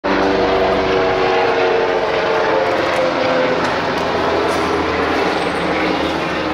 ヘリコプターがオーシャンスタジアム上空を通過 鴨川シーワールドにて 2020年10月24日 00298
ヘリコプターがオーシャンスタジアム上空を通過
2020年10月24日